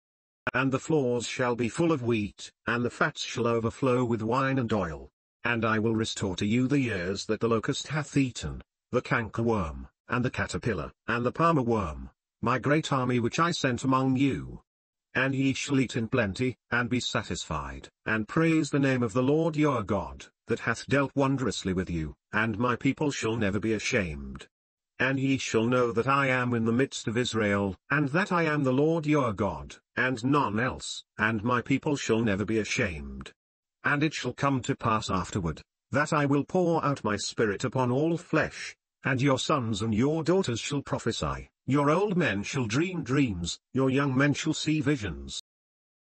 Book of Joel 2:24-28 | King James Bible Narration
24 And the floors shall be full of wheat, and the fats shall overflow with wine and oil.
25 And I will restore to you the years that the locust hath eaten, the cankerworm, and the caterpiller, and the palmerworm, my great army which I sent among you.
26 And ye shall eat in plenty, and be satisfied, and praise the name of the LORD your God, that hath dealt wondrously with you: and my people shall never be ashamed.
27 And ye shall know that I am in the midst of Israel, and that I am the LORD your God, and none else: and my people shall never be ashamed.
28 And it shall come to pass afterward, that I will pour out my spirit upon all flesh; and your sons and your daughters shall prophesy, your old men shall dream dreams, your young men shall see visions:

Source: King James Version Bible, 1611
Images are generated by: Krita 5.2.2. / ComfyUI